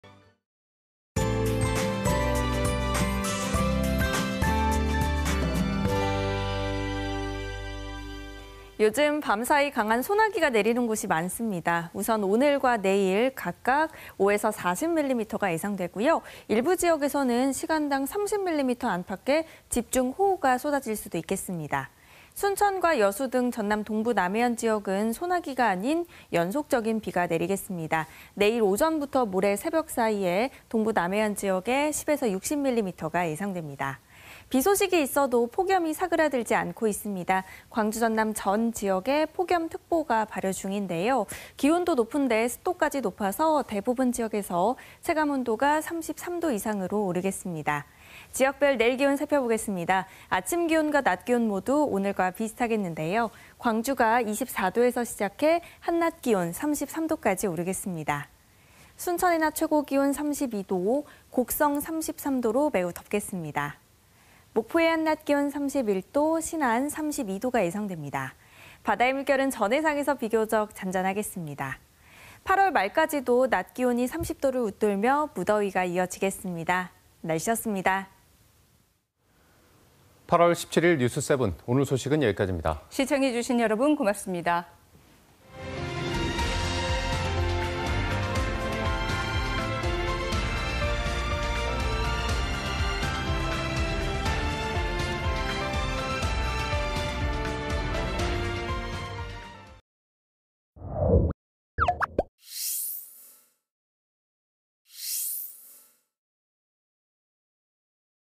[날씨] 광주·전남 강한 소나기 주의…내일도 체감 ‘33도’ 이상 / KBS 2023.08.17.
요즘 밤사이 강한 소나기가 내리는 곳이 많습니다.

우선 오늘과 내일 각각 5~40mm가 예상되고요,

 일부 지역에서는 시간당 30mm 안팎의 집중호우가 쏟아질 수도 있겠습니다.

 순천과 여수 등 전남 동부 남해안 지역은 소나기가 아닌 연속적인 비가 내리겠습니다.

내일 오전부터 모레 새벽까지 흐리고, 10~60mm의 비가 예상됩니다.

 비 소식이 있어도 폭염이 사그라들지 않고 있습니다.

 광주 전남 전 지역에 폭염 특보가 발효 중인데요,

 기온도 높은데, 습도까지 높아서 대부분 지역에서 체감 온도가 33도 이상으로 오르겠습니다.

 지역별 자세한 내일 기온 입니다.

아침 기온과 낮 기온 모두 오늘과 비슷하겠는데요,

광주가 24도에서 시작해 낮 기온 33도까지 오르겠습니다.

 순천의 낮 기온 32도, 곡성 33도로 매우 덥겠습니다.

 목포의 낮 기온 31도, 신안 32도가 예상됩니다.

 물결은 전 해상에서 비교적 잔잔하겠습니다.

 8월 말까지도 낮 기온이 30도를 웃돌며 무더위가 이어지겠습니다.